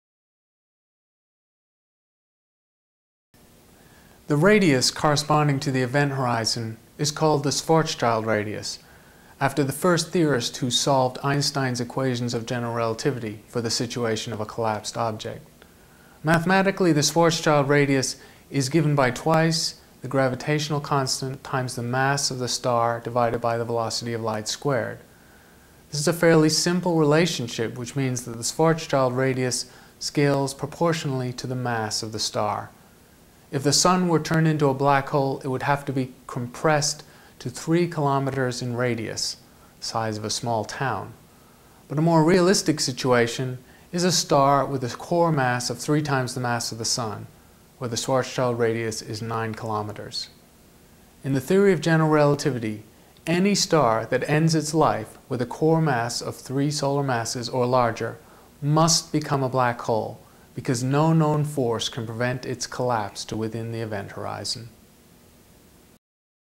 Teach Astronomy - Schwarzschild Radius
The radius corresponding to the event horizon is called the Schwarzschild radius after the first theorist who solved Einstein's equations of General Relativity for the situation of a collapsed object.  Mathematically, the Schwarzschild radius is given by twice the gravitational constant times the mass of the star divided by the velocity of light squared.  This is a fairly simple relationship which means that the Schwarzschild radius scales proportionally to the mass of the star.  If the Sun were turned into a black hole it would have to be compressed to three kilometers in radius, the size of a small town, but a more realistic situation is a star with a core mass of three times the mass of the Sun where the Schwarzschild radius is nine kilometers.  In the theory of General Relativity any star that ends its life with a core mass of three solar masses or larger must become a black hole because no known force can prevent its collapse to within the event horizon.